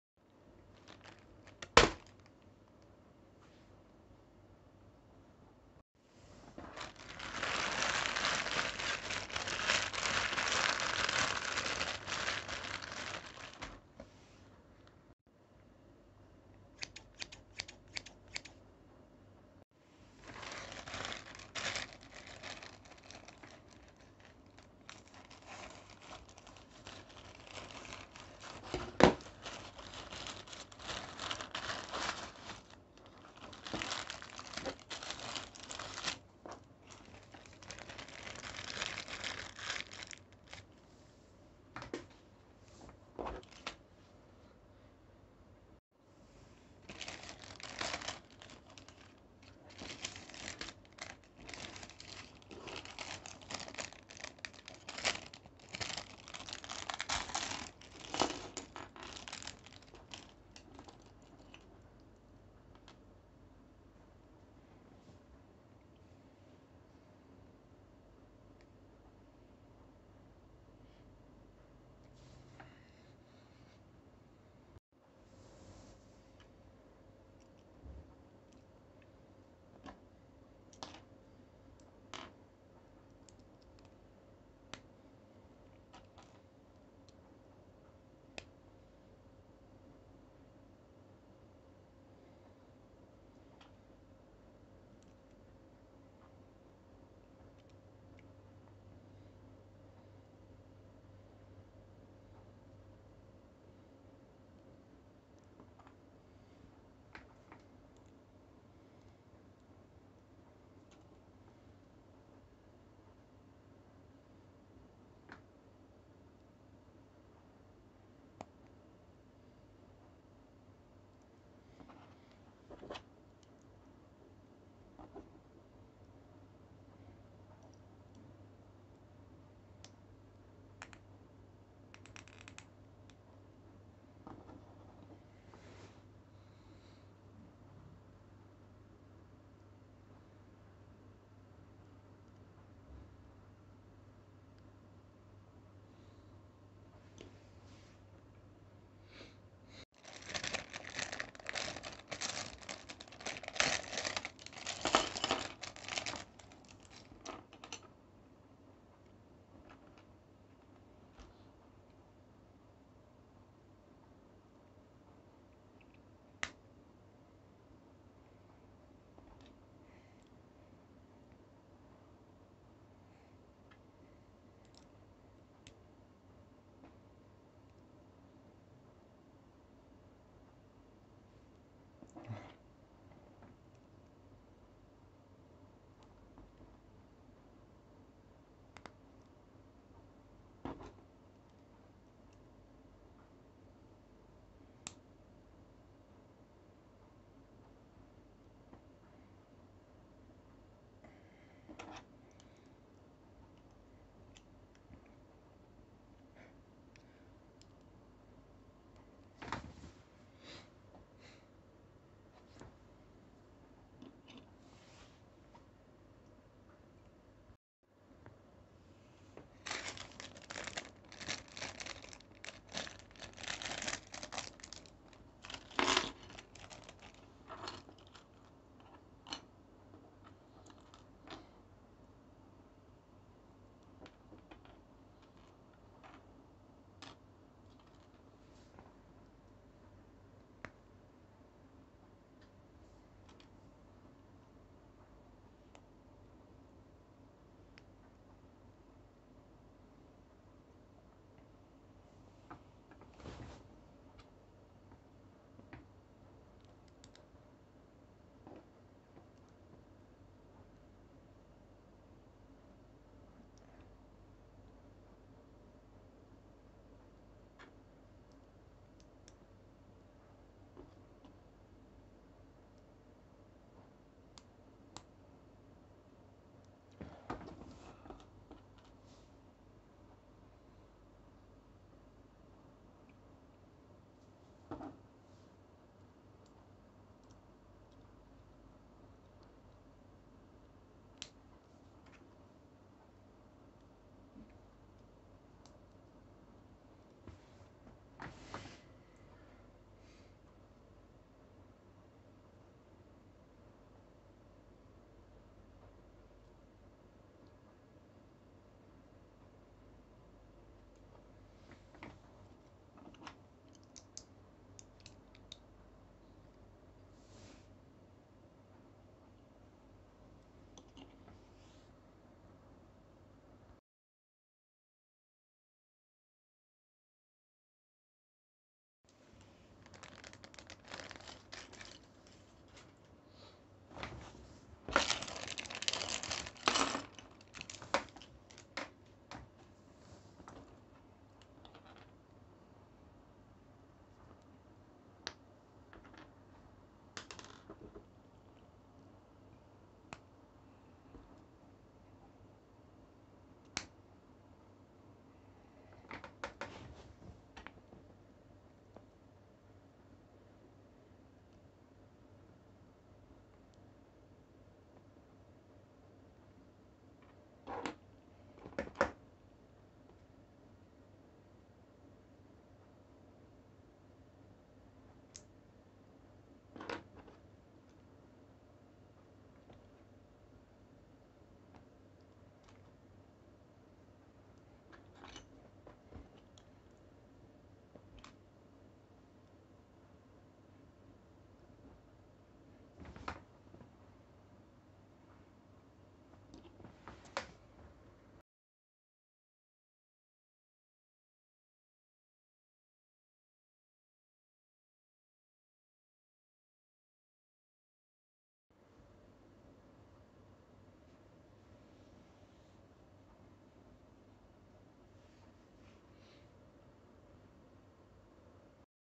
LEGO Samurai & MasterChief review
In this video, we're looking at this Chinese Lego custom Samurai minifigures and a masterchief minfigure from Halo franchise.